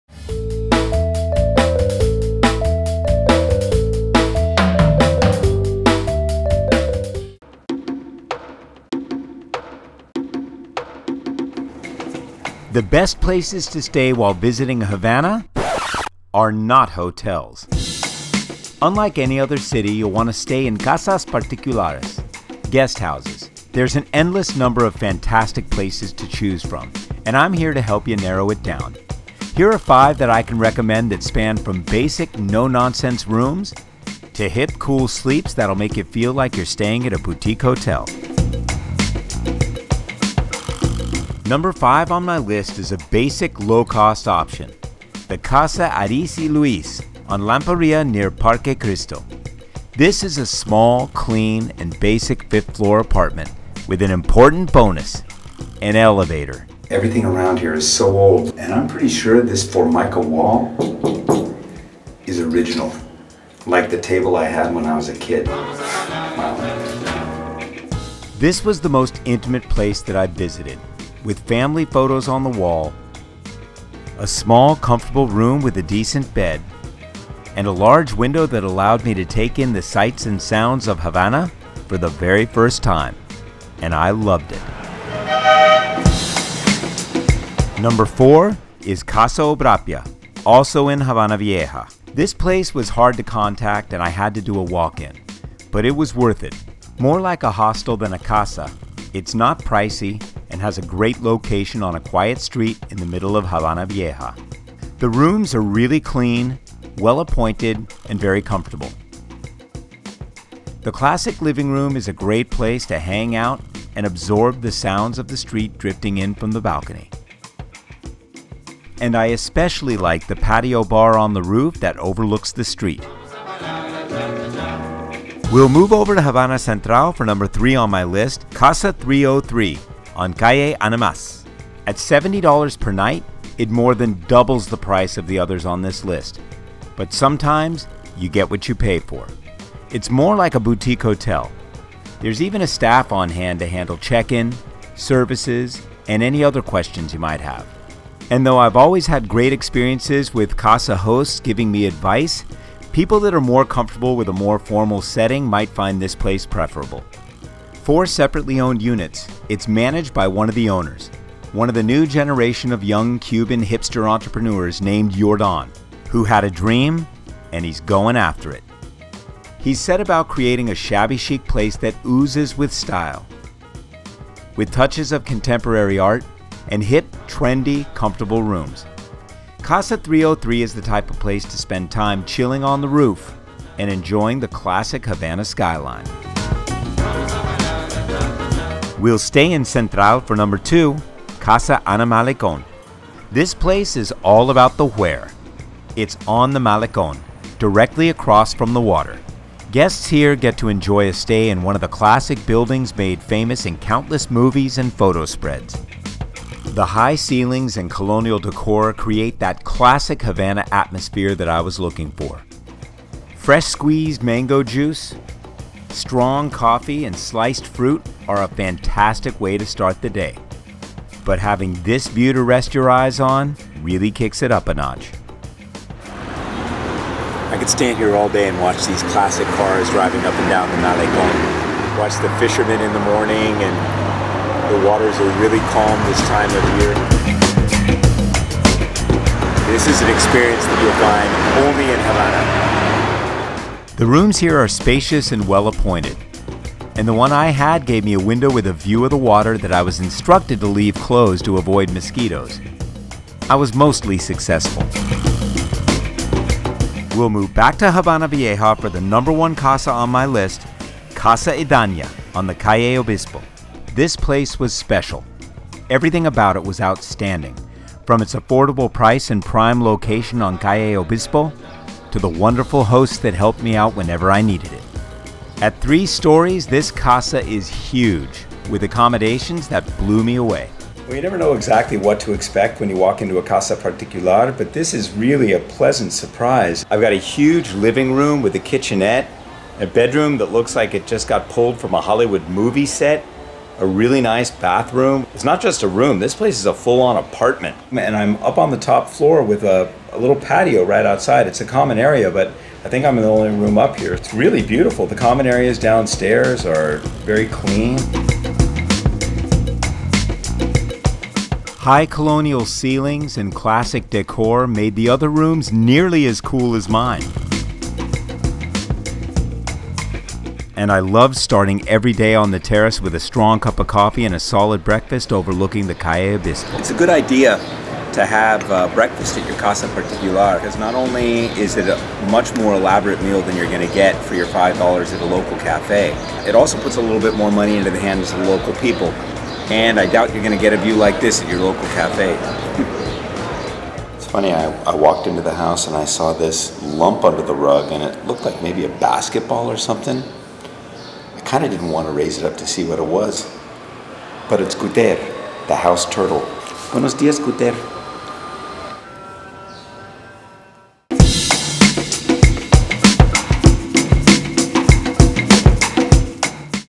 Top 5 Havana Casas Particulares, HD Movie
After two trips to havana and staying in several Casas Particulares, I felt ready to make some recommendations. So here's my list of 5 casas that, in my opinion, most travelers won't be disappointed with.
Cuba is different from most other places you'll travel to in that they've been set up for lodging guests in their homes for decades, as it's it's traditionally been one of the only ways Cuban people can make extra money in their tightly controlled economy.  Because of this, they are experts at providing the accommodations and services most travelers are looking for when visiting their country.
Please keep in mind that this list is NOT definitive, only drawn from my experience, and I welcome all suggestions and discussion about other accommodations.  Also, I limited my casas to the neighborhoods of Central & Vieja because those are MY favorite areas to stay for lot's of reasons I won't go in to here.  I am aware I will draw criticism for that ("What? No Vedado?!"), but I have my own personal choices.  I love Vedado, I just don't choose to sleep there.

At the time I stayed in these casas, they ranged from $23 (Adys y Luis) to $70 (Casa 303), with the others all being in the $31-$34 range.  The prices do vary, depending on the site you choose to book on, so do your research.  The links I'm posting are a place to start.
Info:
Casa Idania:
Casa 303:
Casa 303 now goes by the name Hotel Animas 303 on Booking.com
Casa Obrapia: